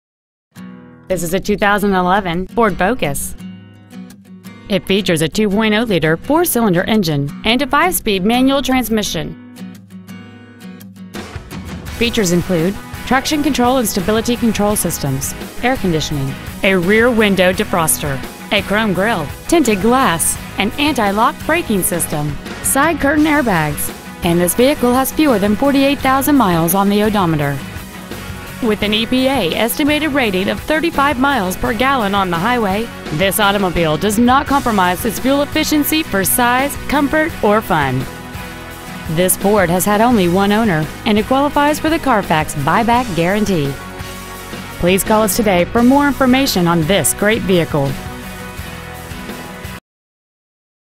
2011 Ford Focus Milford OH
Year : 2011
 Make : Ford
 Model : Focus
 Engine : 2.0l 4 cyls
 Trans . : manual 5-speed
 Exterior : blue flame metallic
 Miles : 47,508
 Interior : medium stone w/cloth sport bucket seats